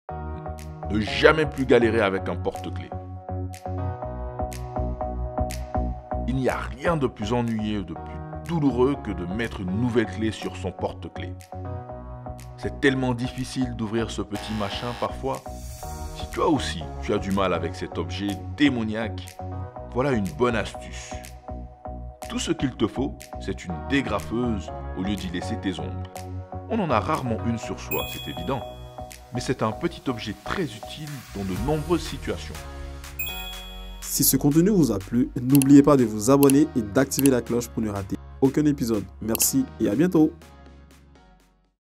Never mess with a key door - Ne jamais galérer avec un porte clé - Astuce Shok
There’s nothing more boring and painful than putting a new key on your key chain. But luckily we have a tip for you.

Il n’y a rien de plus ennuyeux et douloureux que de mettre une nouvelle clé sur son porte clé. Mais heureusement nous avons une astuce pour toi.